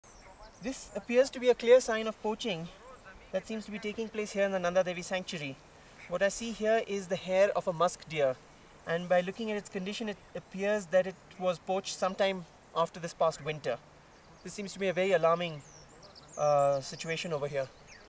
Hair of poached Musk Deer
These Musk Deer were found poached inside the Nanda Devi Sanctuary, Uttarakhand, India.

Musk deer are artiodactyls of the genus Moschus, the only genus of family Moschidae.

The Musk perfume is now manufactured in the laboratory as Astrolide (Galaxolide equivalent) and one does not need to harvest Musk pods for the musk fragrance. The pod of the Musk Deer sells for tens of thousands of dollars by the kilogram, and it is rumoured that Chanel uses it for its popular perfume Chanel No. 5. It has not been proved as although Wilderness Films India research in the past has found strong evidence to confirm this conjecture. International pressure has perhaps forced them to replace the genuine musk (for which animals need to be killed) with the chemical equivalent of it - Astrolide!

They are more primitive than the cervids, or true deer, in not having antlers or facial glands, in having only a single pair of teats, and in possessing a gall bladder, a caudal gland, a pair of tusk-like teeth and of particular economic importance to humans a musk gland. Moschids live mainly in forested and alpine scrub habitats in the mountains of southern Asia notably Himalayas. Musk deer resemble small deer with a stocky build, and hind legs longer than their front legs. They are approximately 80-100 cm in length, 50-70 cm tall at the shoulder, and weigh between 7 and 17 kg. The feet of musk deer are adapted for climbing in rough terrain. The musk gland is found only in adult males. It lies in a sac located between the genitals and the umbilicus, and its secretions are most likely used to attract mates. Musk deer are herbivores, living in hilly, forested environments, generally far from human habitation. Female musk deer give birth to a single fawn after about 150-180 days. The newborn young are very small, and essentially motionless for the first month of their life, a feature that helps them remain hidden from predators.

Source: Wikipedia